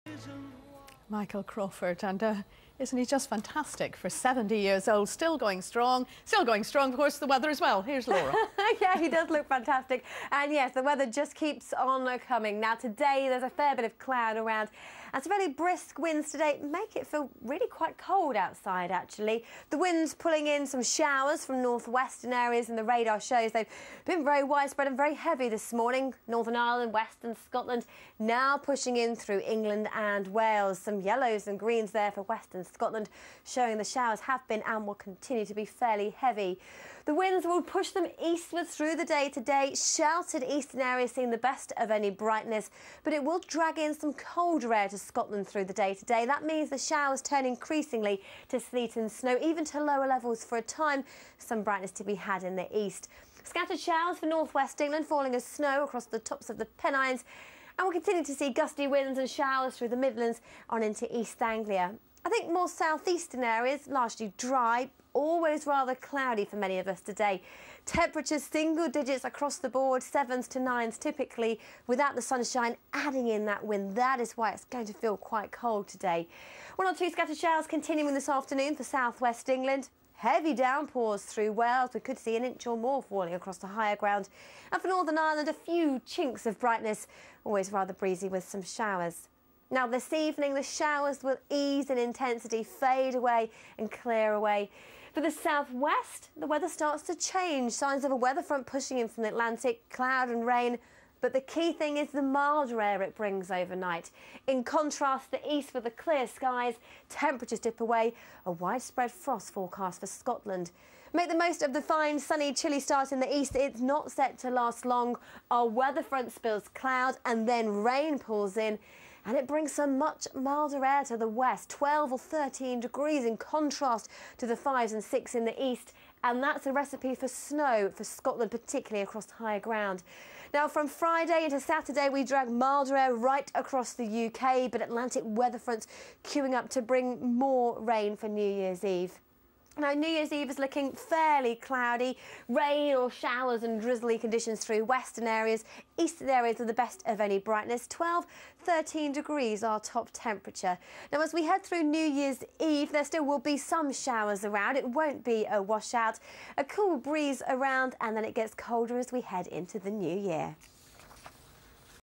laura tobin -378
Laura tobin looking absolutely stunning this morning.
I may have missed one forecast but will eventually upload anymore from this week.